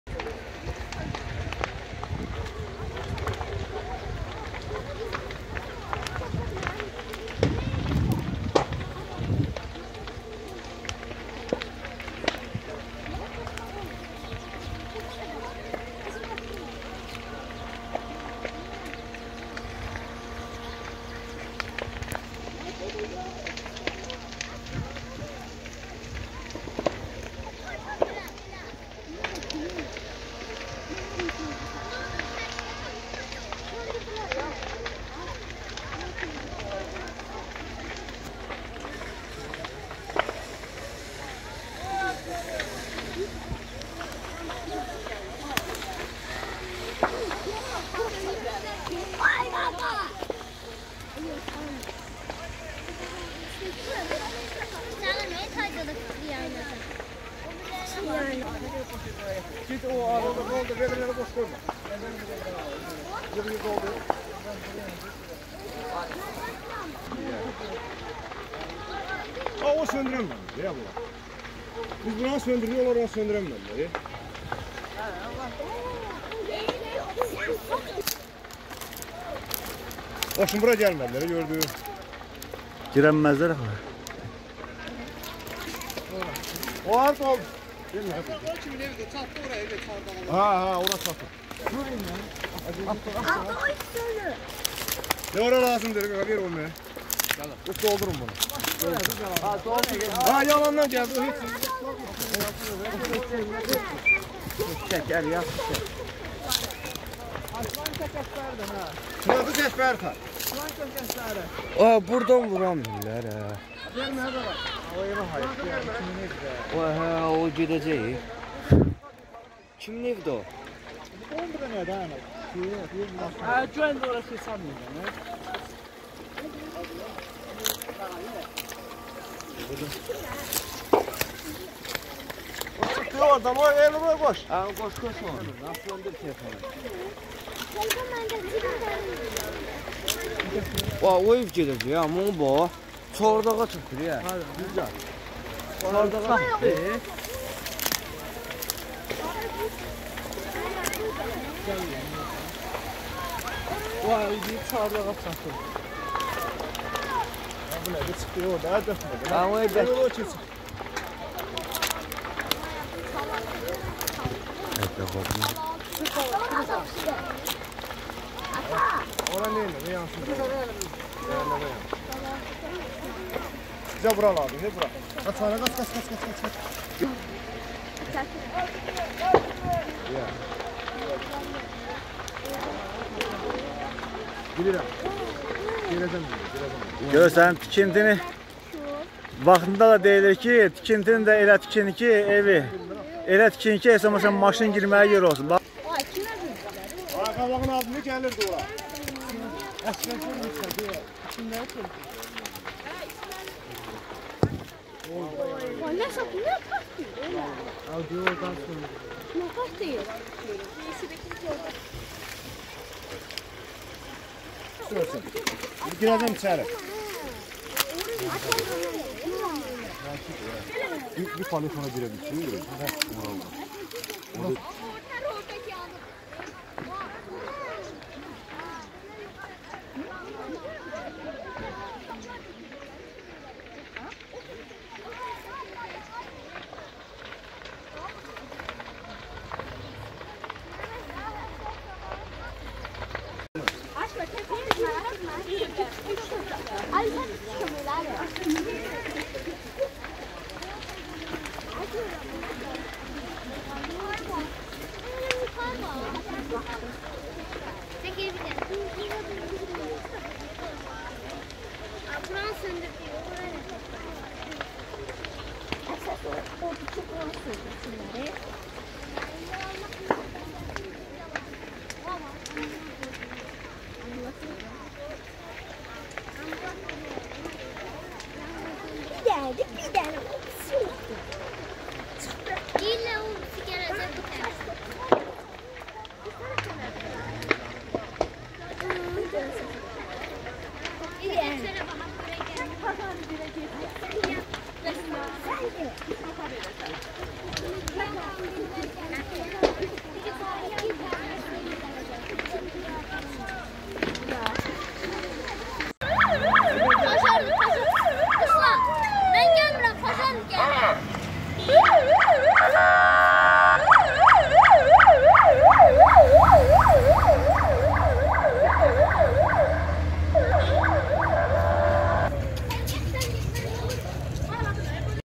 İmişlidə güclü yanğın başlayıb
İmişli rayonu, Pənah Həsənov küçəsində güclü yanğın başlayıb. Yanğın böyük qamışlıq ərazini əhatə edir. Hadisə fərdi yaşayış evlərinin yaxınlığında baş verib. Yanğın evlərə yaxınlaşır. Əraziyə FHN-in yanğınsöndürən texnikaları və əməkdaşları cəlb edilib. Hazırda həm FHN-in qüvvələri, həm də ətrafda yaşayan sakinlər yanğının söndürülməsi üçün çalışırlar.